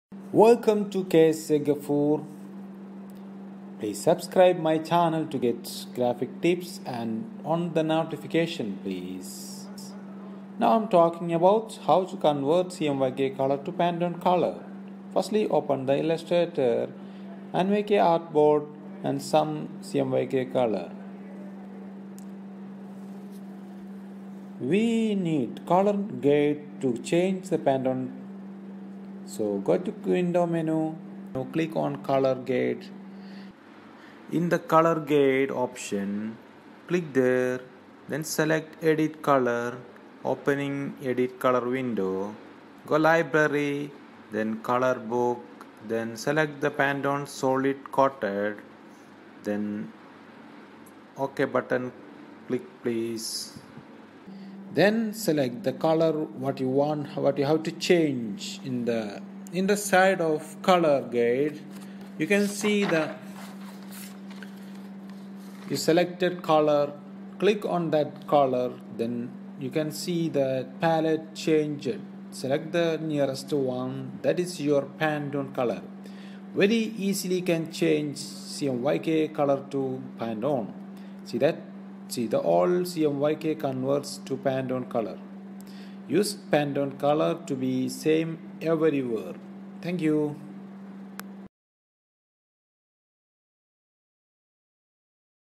Convert CMYK Color to Pantone Color | Pre Press | Graphic Design | Adobe Illustrator
How to convert CMYK color to Pantone in Adobe Illustrator

In this video I am going to demonstrate a quick way you can convert CMYK colour swatches into Pantone swatches in Adobe illustrator.

Easy convert CMYK color to Pantone color.